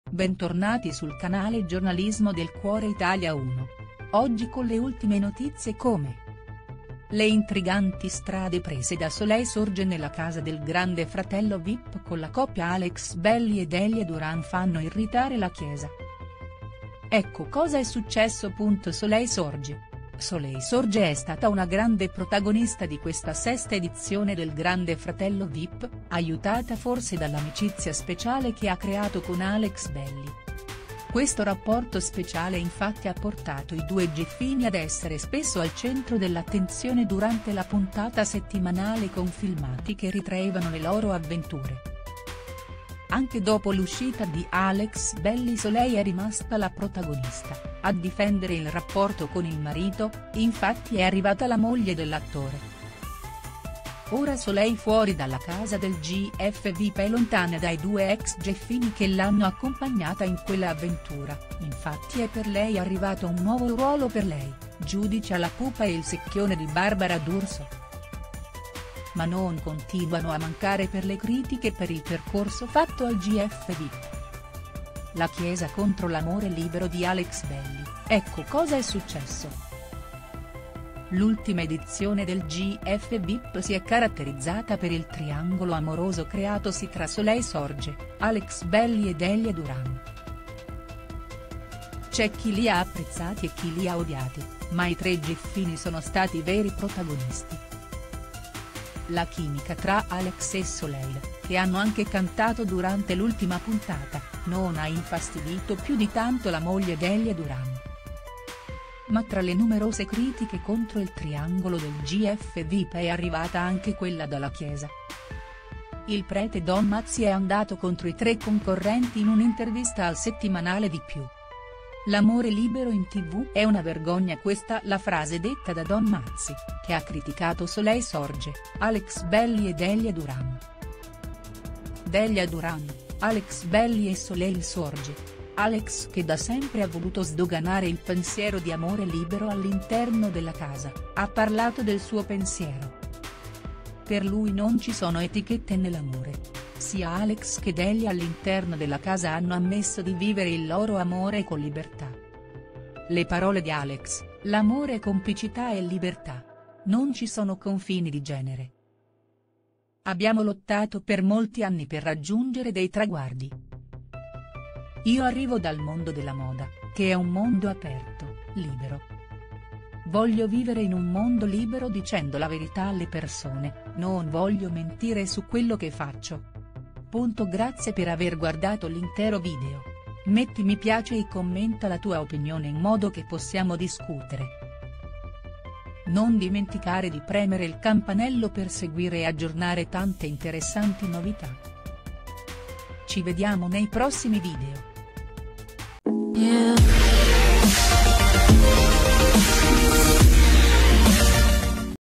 Soleil Sorge scomunicata dalla Chiesa “Vergogna” Tutta colpa di Alex Belli
Soleil Sorge è stata una grande protagonista di questa sesta edizione del Grande Fratello Vip, aiutata forse dall’amicizia speciale che ha creato con Alex Belli. Questo rapporto speciale infatti ha portato i due gieffini ad essere spesso al centro dell’attenzione durante la puntata settimanale con filmati che ritraevano le loro avventure.

Anche dopo l’uscita di Alex Belli Soleil è rimasta la protagonista, ad difendere il rapporto con il marito, infatti è arrivata la moglie dell’attore. Ora Soleil fuori dalla casa del GF Vip è lontana dai due ex gieffini che l’hanno accompagnata in quella avventura, infatti è per lei arrivata un nuovo ruolo per lei, giudice a La Pupa e il Secchione di Barbara D’Urso. Ma non continuano a mancare per le critiche per il percorso fatto al GF Vip.